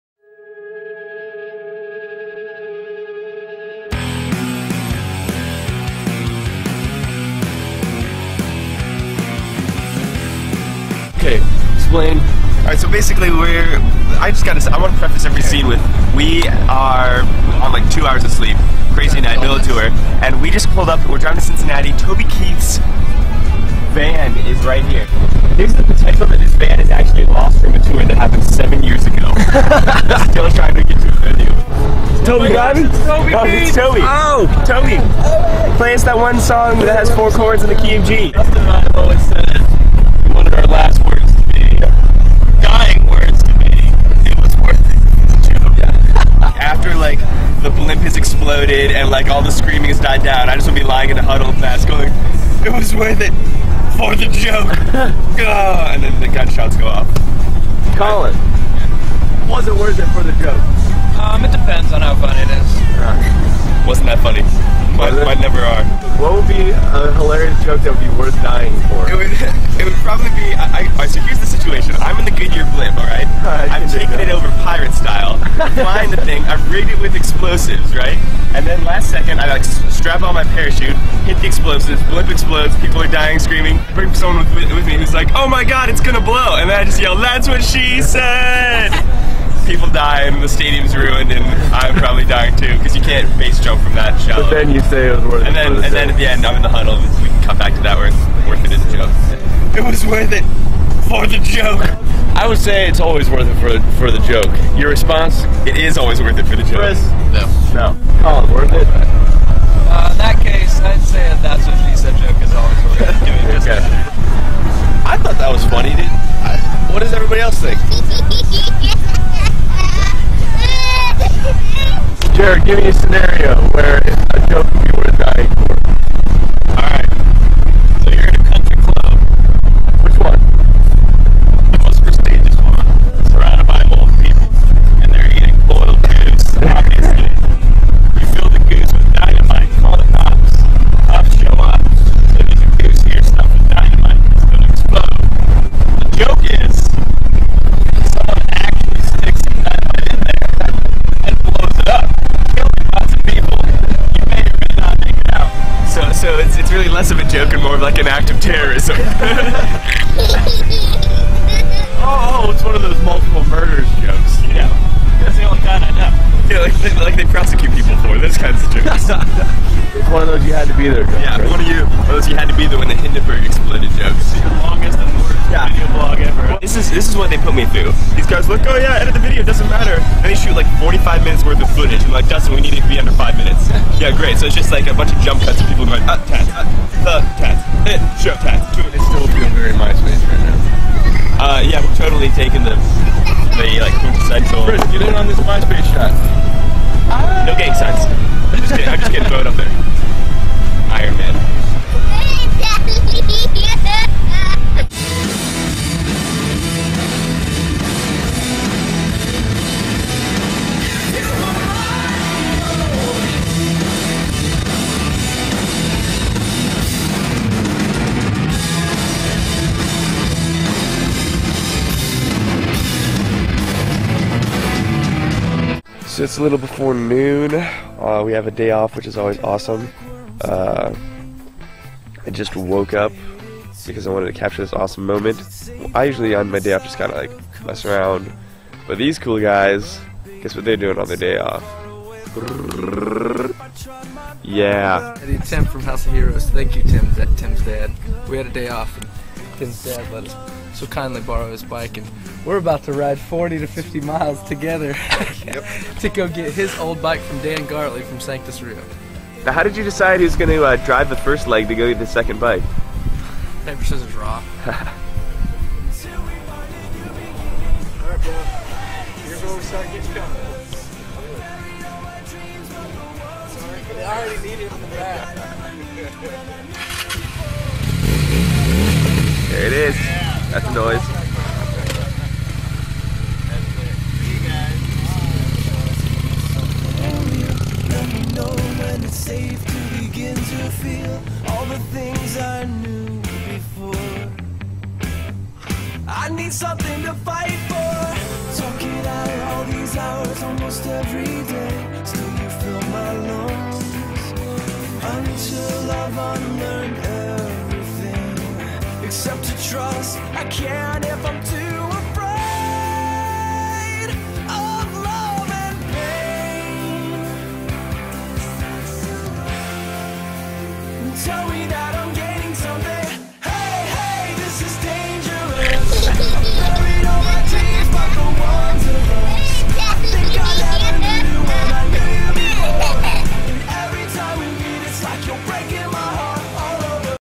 House of Heroes SBATCT Vlog 4
The fantastic fourth installment of the Super Bigtime American Traveling Concert Tour, brought by you by House of Heroes — recently voted America's most historically accurate band.